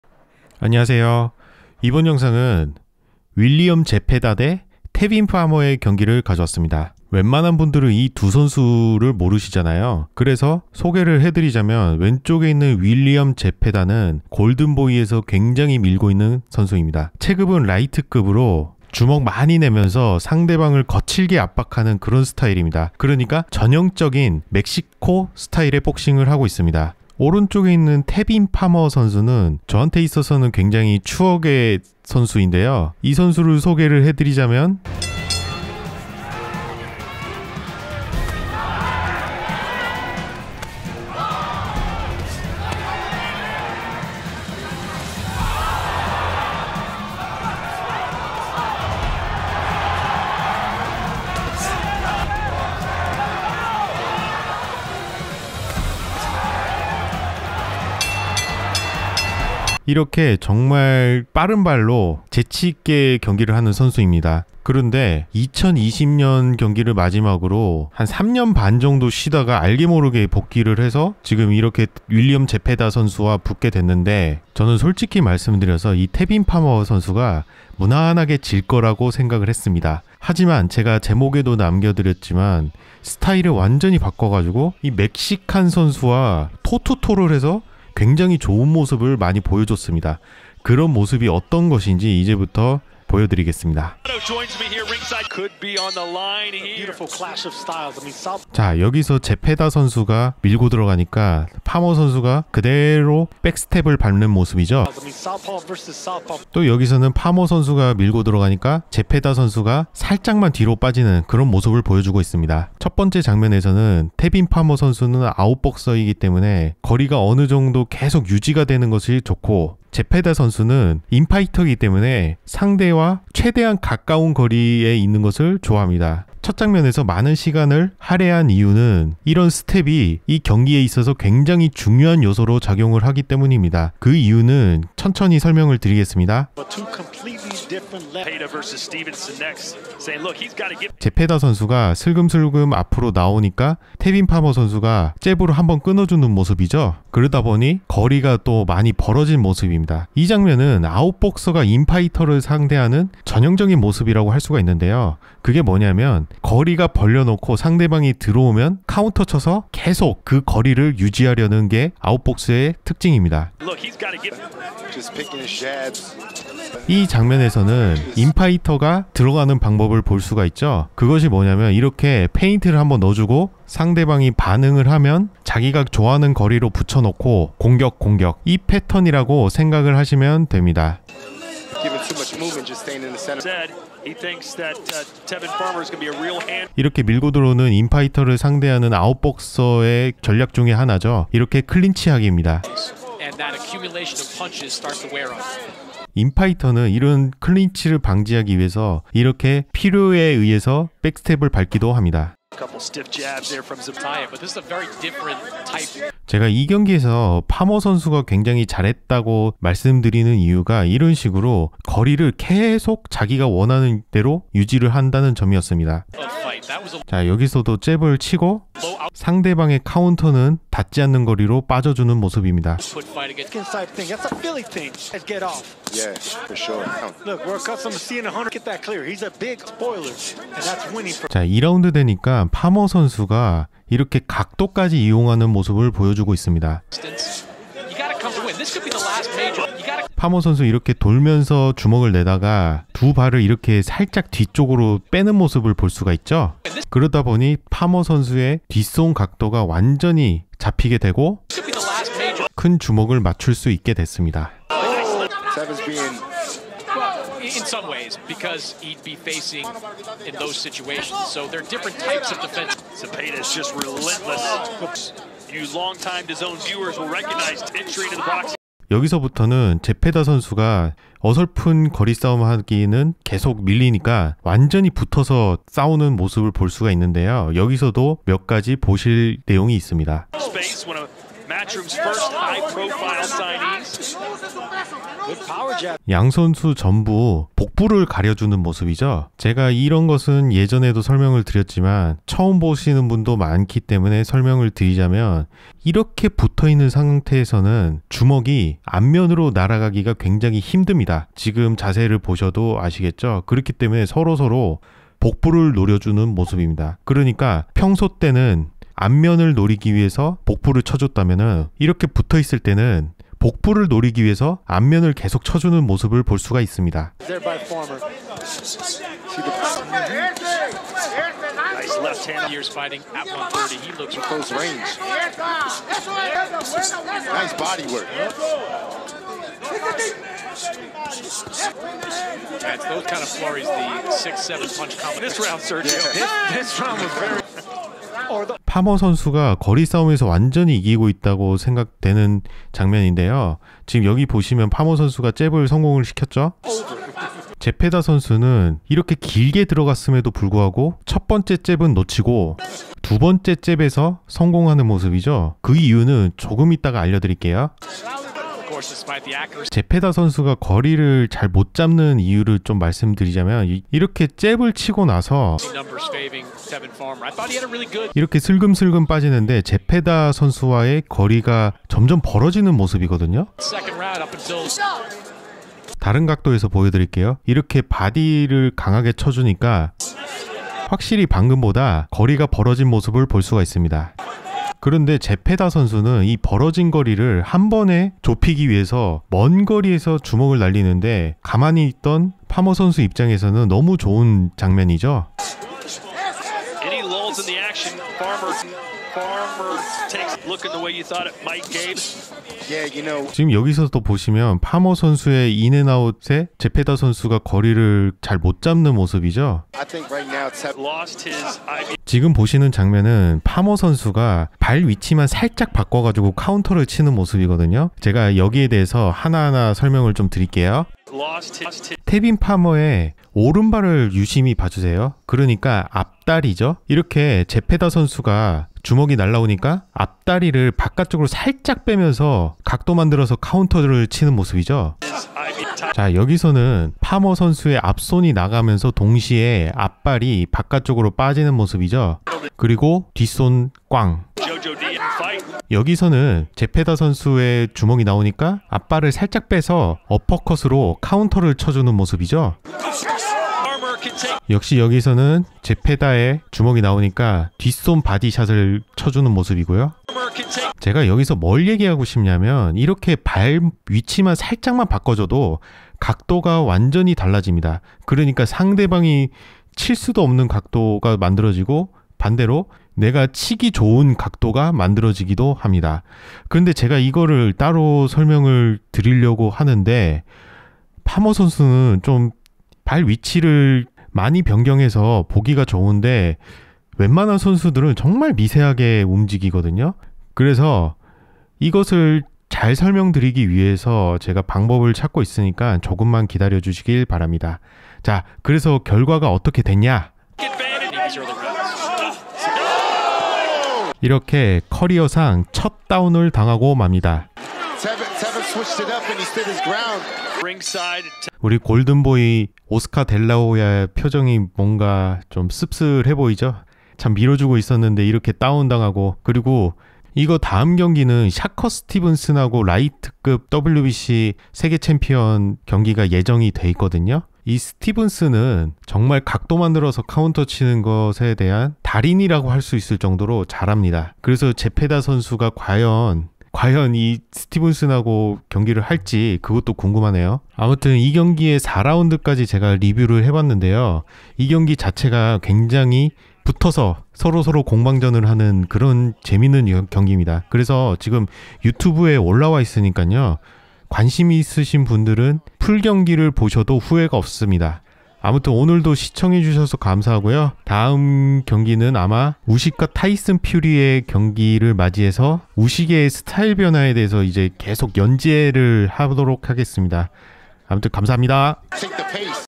멕시칸 복서에게 인파이팅 강의한 아메리칸 아웃 복서 ( 제페다 vs 파머)
2024년 11월 17일 한국 시간으로 새벽에 한 윌리엄 제페다 vs 테빈 파머의 경기입니다.
'윌리엄 제페다'는 라이트급 강자로 WBC의 지명에 의해 '샤커 스티븐슨'과의 대결을 앞두고 있습니다.
그 전에 테스트 경기로 테빈 파머와 경기를 하였는데 ...
생각보다 제페다가 고전한 경기 입니다.
복싱에서 붙어서 싸우는 것에 대한 내용을 경기 초반만 다뤘습니다.
풀경기 영상 보시길 추천드립니다.